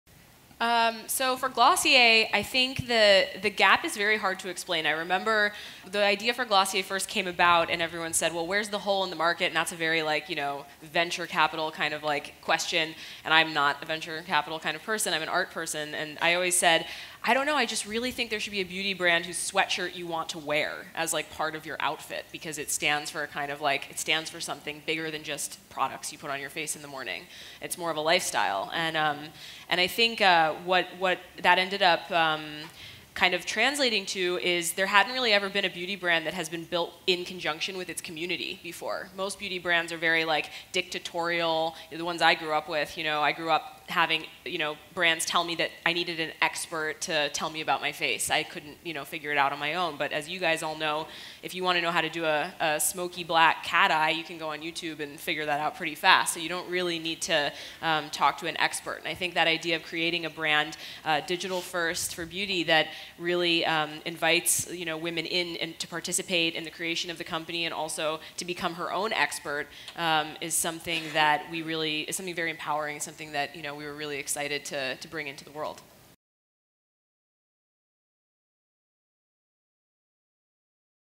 Glossier Founder Emily Weiss on the "Glossier Gap"
At the Los Angeles Girlboss Rally, 2017.